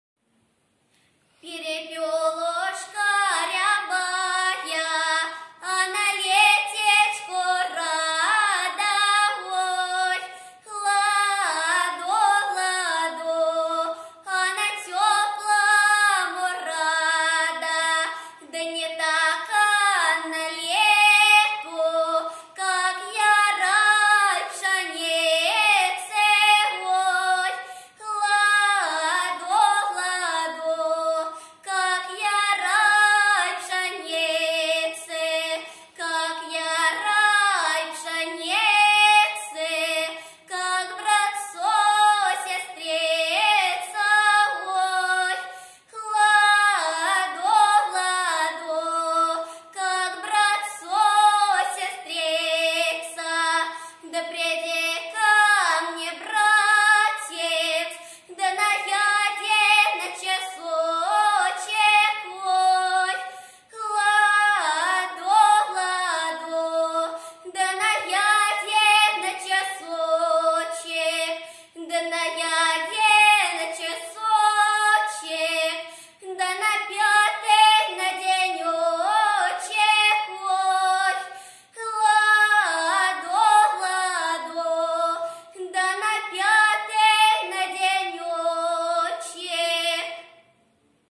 Бескоровайная Ангелина Игоревна (12 лет)
Бескоровайная Ангелина Игоревна (12 лет), преподаватель Солодовникова Маргарита Владимировна, Курская область, г. Курск, ОБПОУ "Курский музыкальный колледж имени Г.В. Свиридова" Отделение дополнительного образования детей (музыкальная школа)